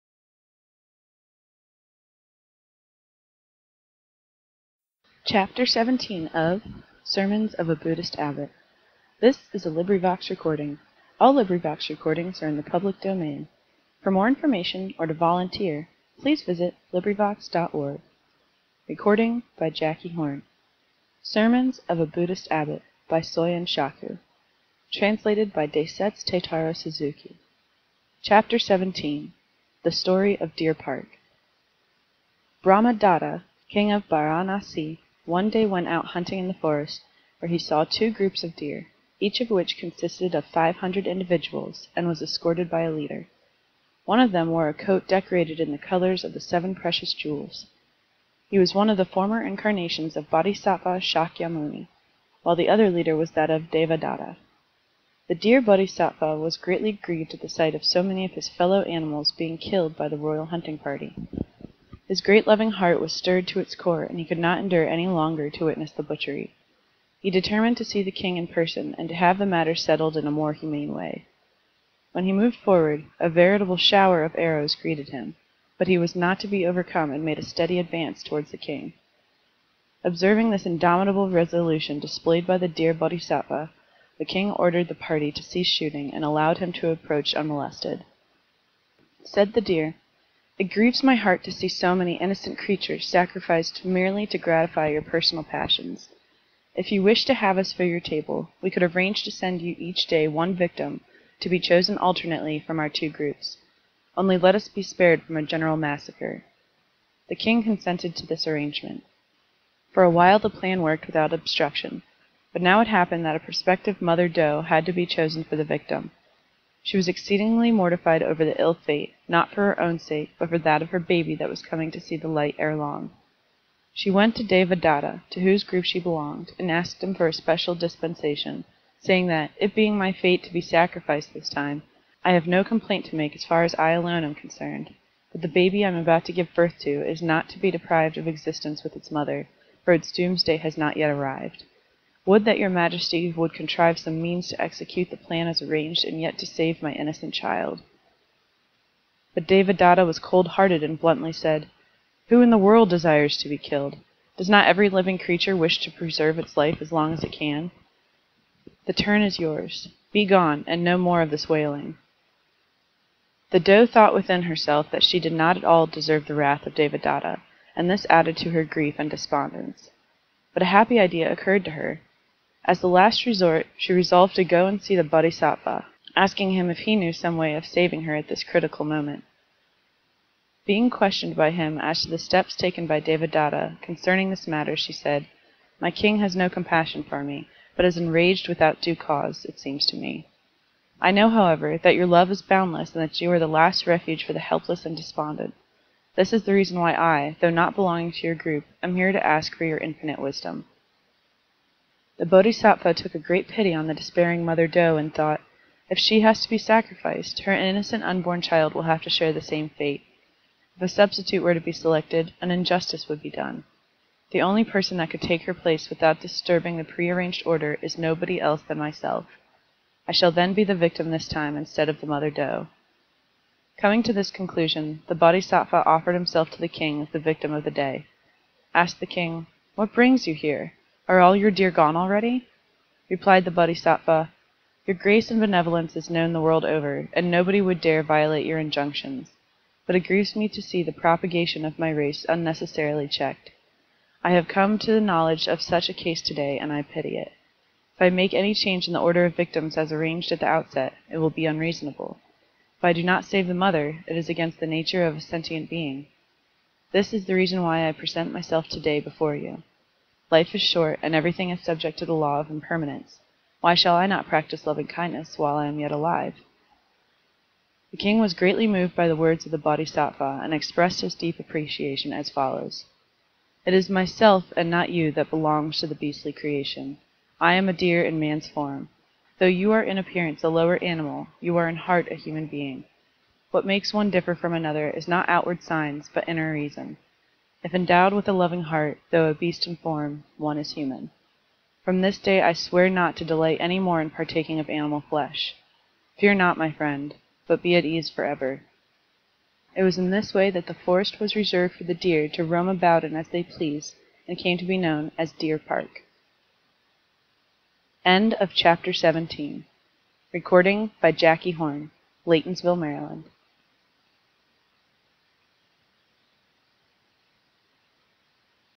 Sermons of a Buddhist Abbot by Soyen Shaku - 17/22. The Story of Deer Park
LibriVox recording of Sermons of a Buddhist Abbot by Soyen Shaku. (Translated by Daisetz Suzuki.) Read in English by Jendia; Mike Pelton; Jacki Horn; csheeler; William Gavula.

Cover image: "A Fairy Moon and a Lonely Shore," Matsumoto print (woodcut), collection of Library of Congress. CD cover design by LA Walden. This design is in the public domain.